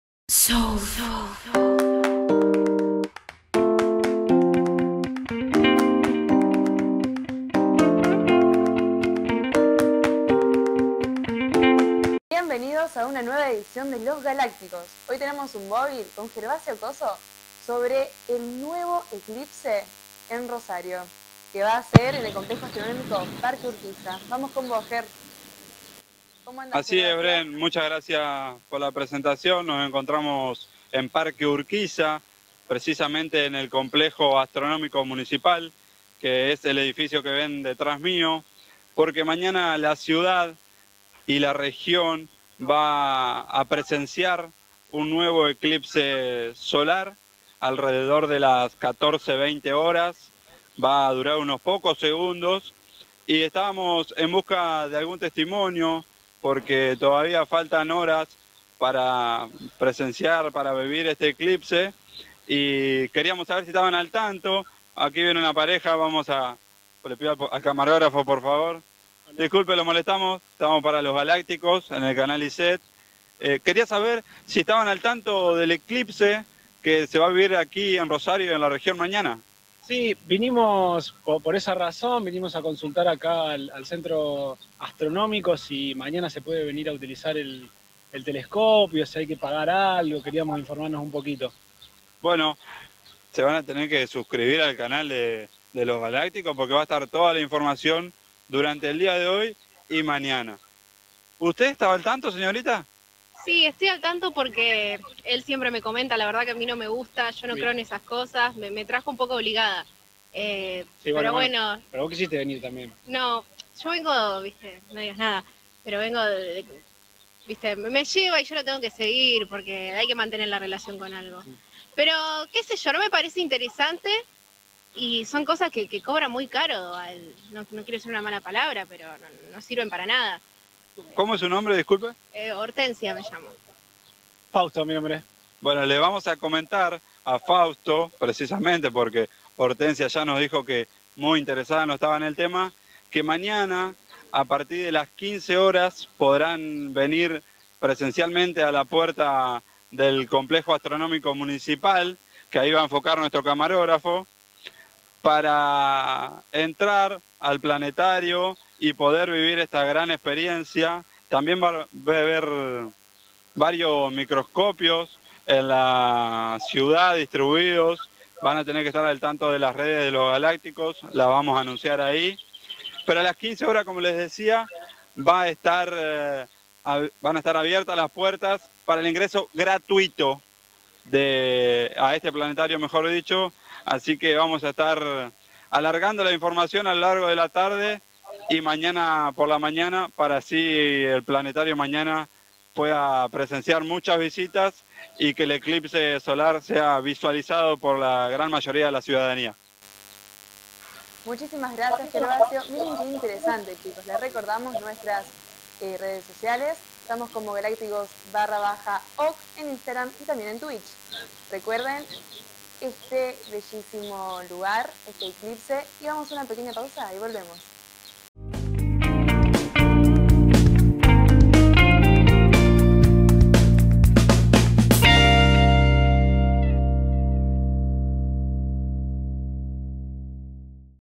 2024 - Móvil de exteriores de TV - Gervasio Cosso
Actividad dentro del dictado de la materia Práctica Integral de TV 1